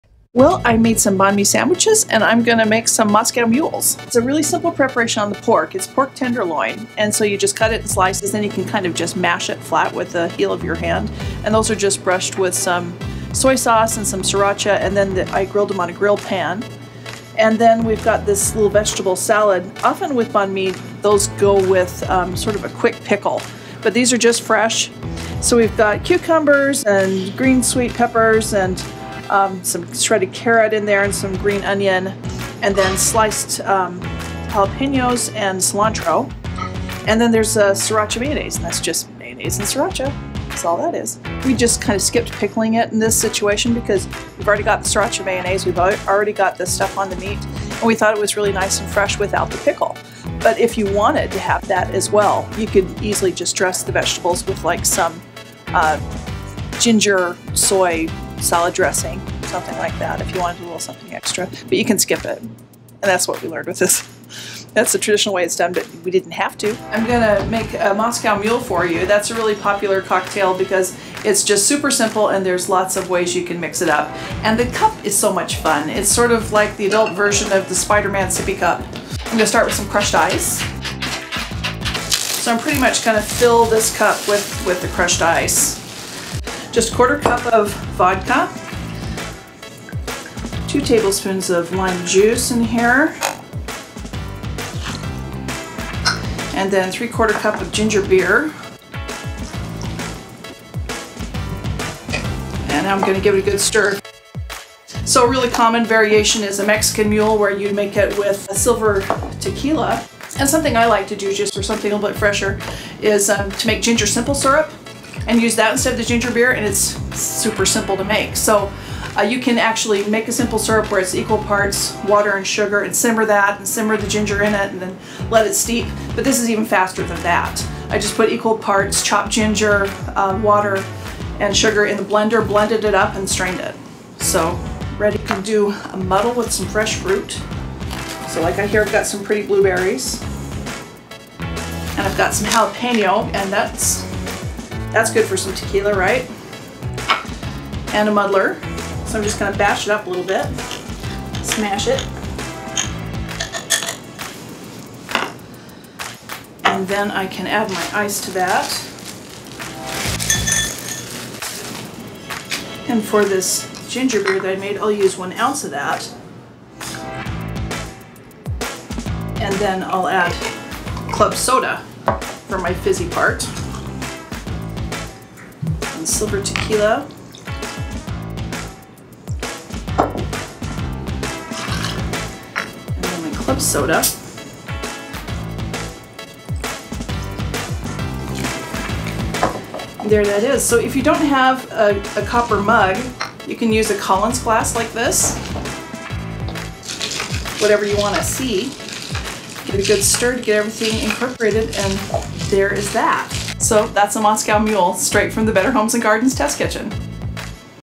Make this trendy sandwich paired with a classic cocktail
A Better Homes and Garden culinary specialist shows how to make a Bahn mi sandwich as well as a Moscow mule with a twist.